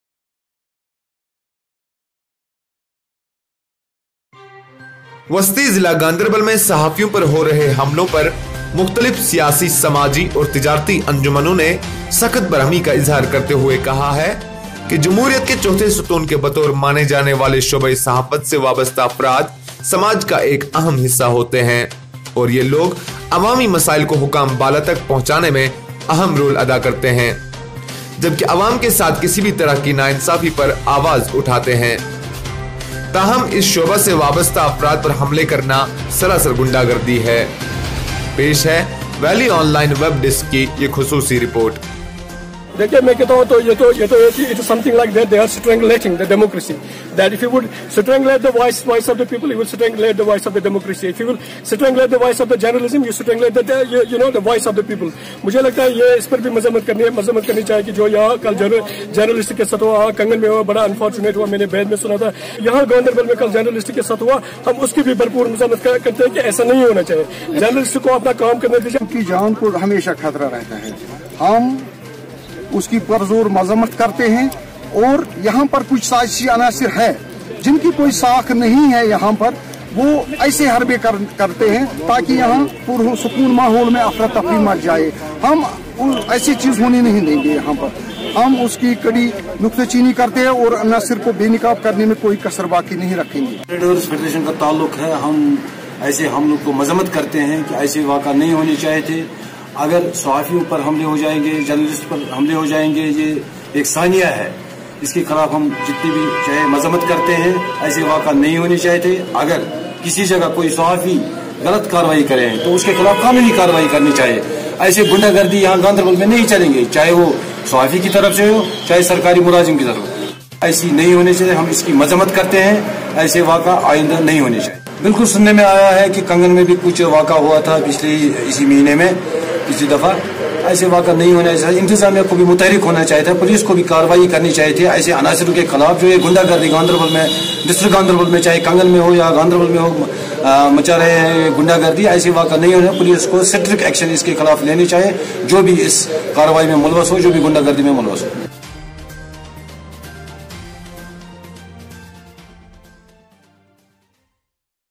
Attack on Journalists continues in Ganderbal, two more reporters thrashed, politicians, social activ
Attack on Journalists continues in Ganderbal, two more reporters thrashed

GANDERBAL: Just after few weeks when a Journalist was physically assaulted
in Kangan Ganderbal, Another incident come to fore on saturday when two more journalist were thrashed and threatned by an employee of labour department in Ganderbal.

Two Journalists working with local daily Newspaper and News Agency said that they were doing their professional job after they recived some complaints regarding slow service in assistant labour Commissioner office Ganderbal.

They said we went to the office and talked to Assistant Labour Commissioner about the complaints received from labourers.

After leaving from the office they were allegedly followed by an employee working in ALC office Ganderbal.

They were grabbed by the employee on road and took them in a restaurant and thrashed them mercilessly.

They also received threats from the employee.

The journalists said that the employee physically assaulted them and ironically made baseless allegation against them.

"The journalists fraternity in Ganderbal are feeling insecure, We are being targeted again and again only for doing our professional duties. We are being intimated to remain silent. But we will continue to work irrespective of threats" A senior journalist told Valley Online.

Ganderbal Journalist association has condemned the incident and demanded strict disciplinary and legal action against the employees